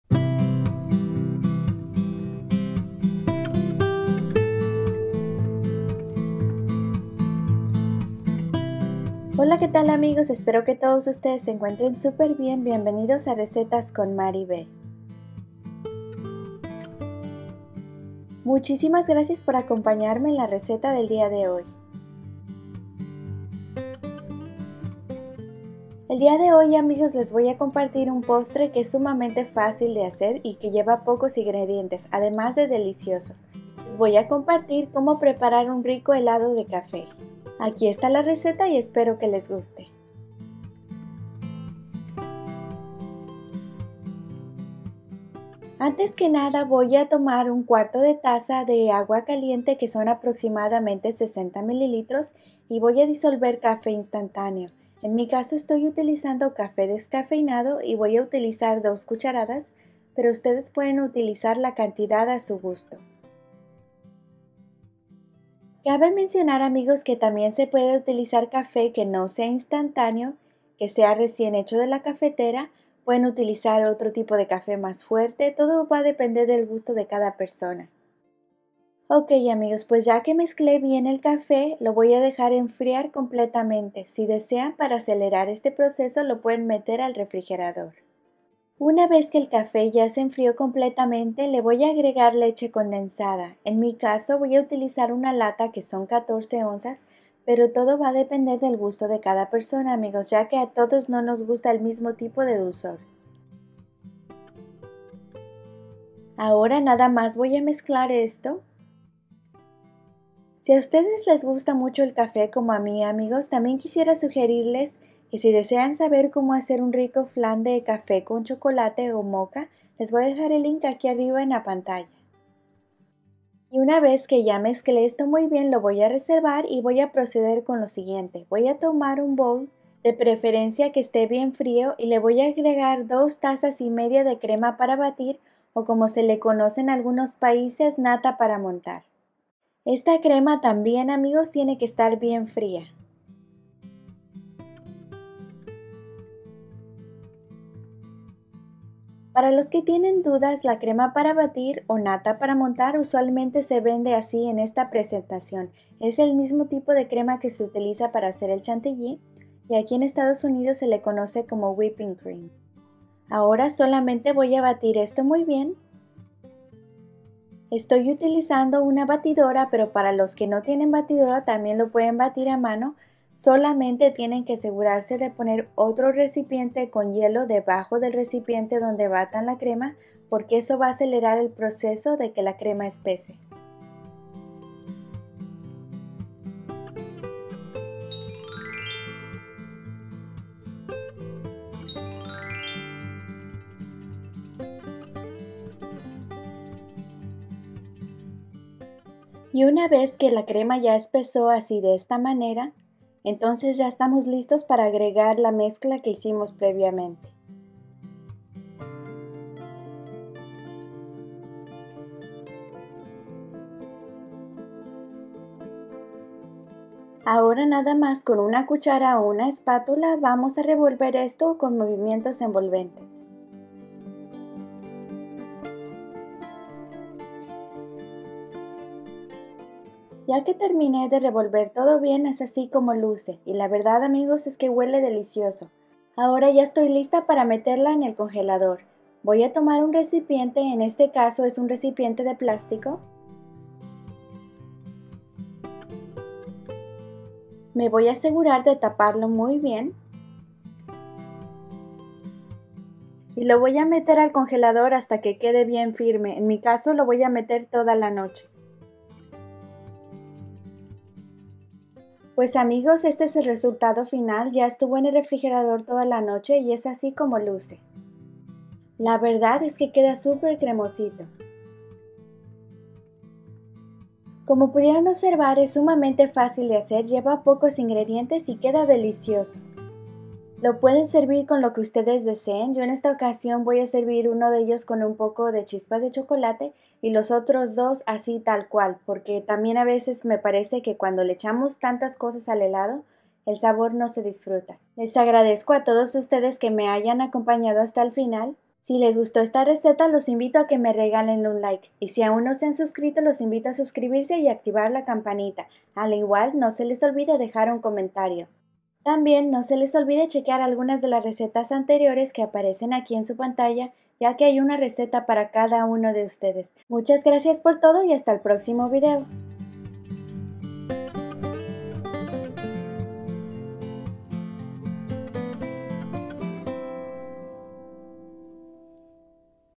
COMO HACER HELADO DE CAFÉ
Este helado de café con leche condensada es muy facil y lleva pocos ingredientes. Es muy delicioso también. Recuerden que pueden modificar la cantidad de café y leche condensada a su gusto.